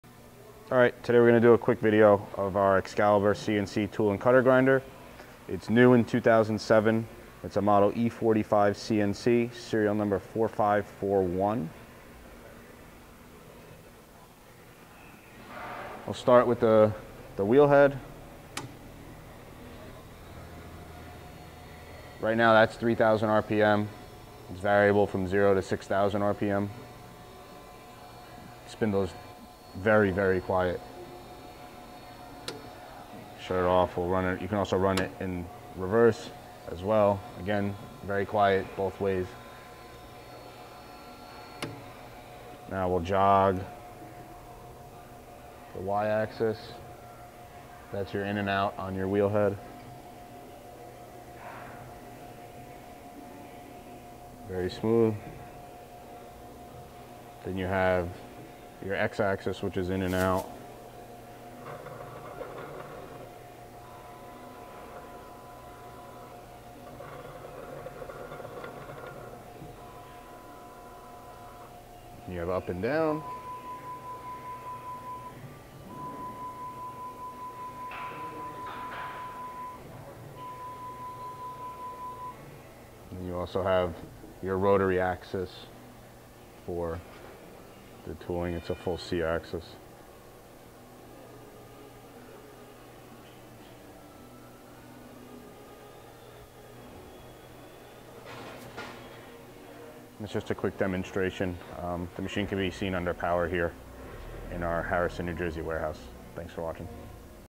EXCALIBUR CNC TOOL & CUTTER GRINDER
New: 2007

The E45 is designed to manufacture small end mills from solid blanks
 in ONE chucking.  It has four CNC axes and one pneumatic axis.
 The E45 has superb accuracy to produce tools from 1/32" to 3/4" diameter,
 straight or helical flute to 60 degree spiral.

MODEL: E45CNC    S/N: 4541     NEW: 2007

Max Tool Diameter: 0.750", 19.05 mm
Min Tool Diameter: 0.030", 0.76 mm
Max Length of Grind: 3.0", 76.2 mm
Max Overall Length of Tool: 8.0", 304.8 mm

WHEELHEAD AXES:
Y Slide: 9.0" (228.6 mm), repeatability 0.0002"
C Rotary (pneumatic): 0 to 60º left or Right

TOOLHEAD AXES:
X Slide: 15" (381 mm), repeatability 0.0002"
Z Slide:7.0" (177.0 mm), repeatability 0.0002"
A Rotary : (Work Spindle) free rotation, resolution 0.005º

FEEDRATES:
All Linear Axes: 0-200 IPM
Rotary A Axis: 150 RPM
Rotary C Axis: Pneumatic

WHEELHEAD:
Wheel Spindle RPM: 0-6000 RPM
Wheel Diameters: 4"-6"
Spindle HP: 5 HP (10 HP optional)

CONTROL:
Intel Pentium controlled 6-axis CNC controller.
Digital AC servos drive all linear axes.
Excalibur Conversational tool programs, written in-house by Excalibur.
Optional programs available; Stepdrills, Reamers, Routers, Tapered Mills.

*** EXCEPTIONAL CONDITION - JUST BEAUTIFUL CONDITION **
** INSEPRCT UNDER POWER IN OUR HARRISON, NJ WAREHOUSE **